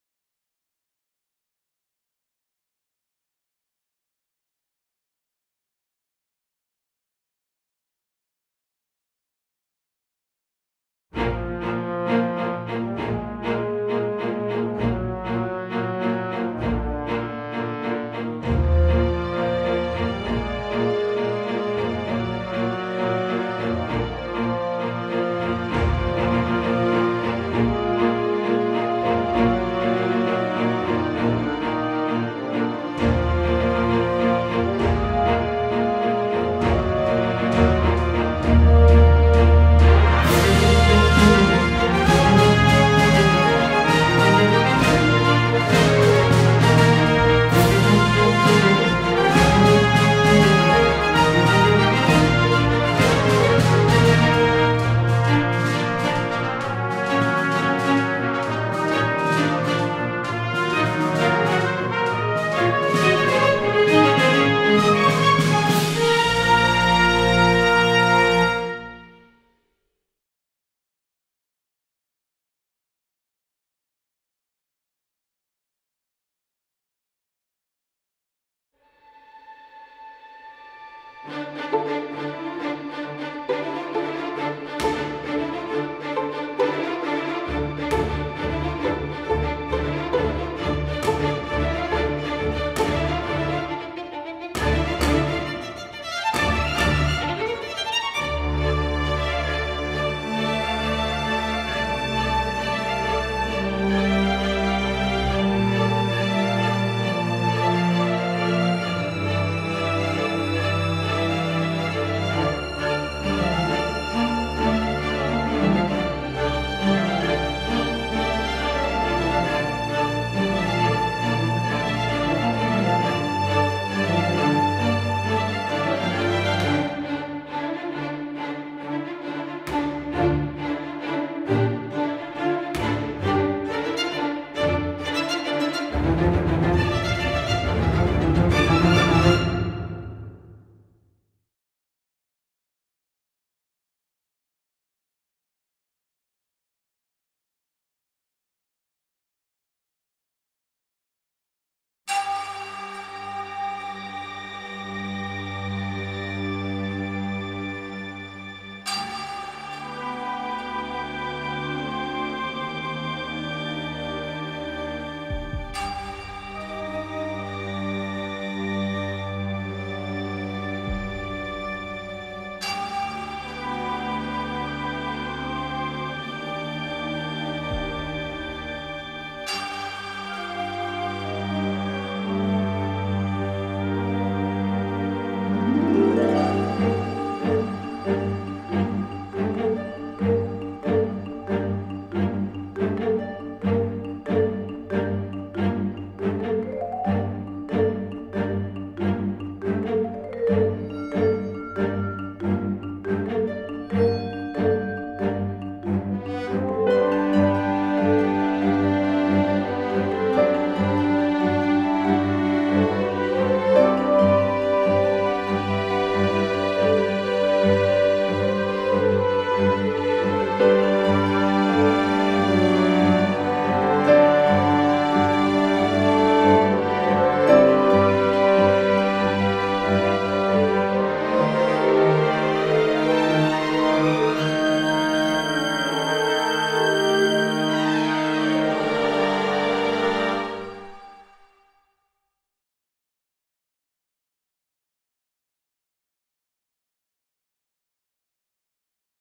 Oticons Faculty 2022 | Film scoring Competition | Enrico Basso
Three tasks of Oticons international film scoring competition 2022.
Music composed and orchestrated by Enrico Basso.